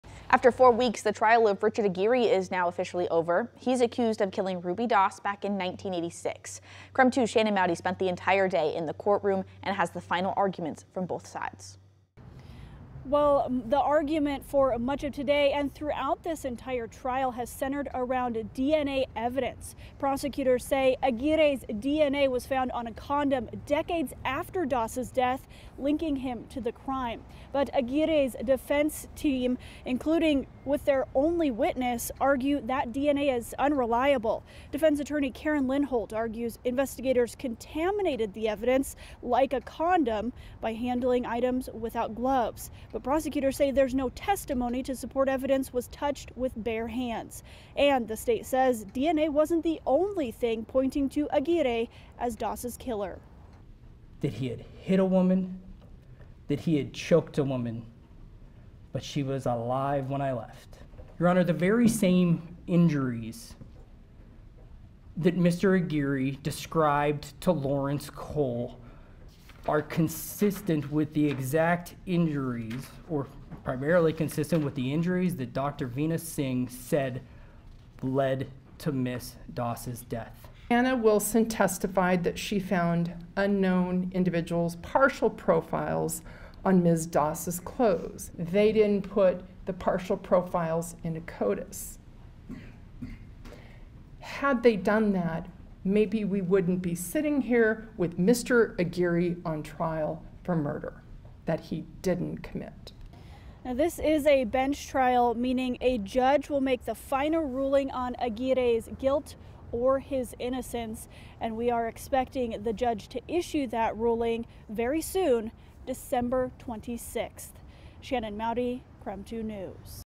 Trial of Richard Aguirre comes to an end, both sides give closing arguments
Aguirre is accused of killing Ruby Doss in 1986. Much of the argument Tuesday and throughout the trial revolved around DNA evidence.